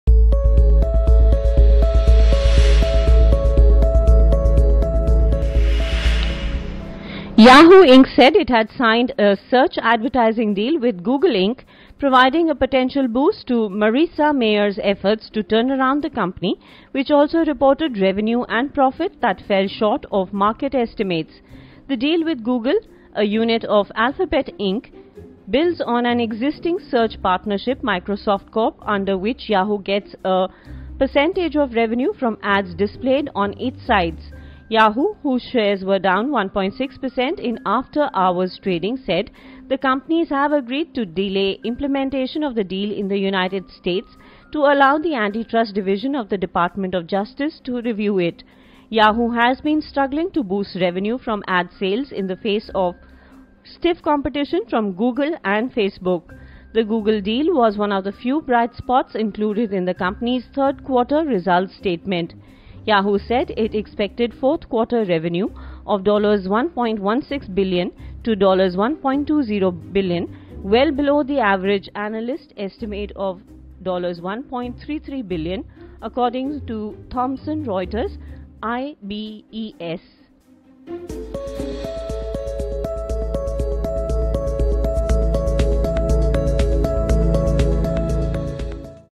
Yahoo seeks Help From Google as Revenue Misses Target
Yahoo Inc said it had signed a search advertising deal with Google Inc, providing a potential boost to Marissa Mayer's efforts to turn around the company, which also reported revenue and profit that fell short of market estimates.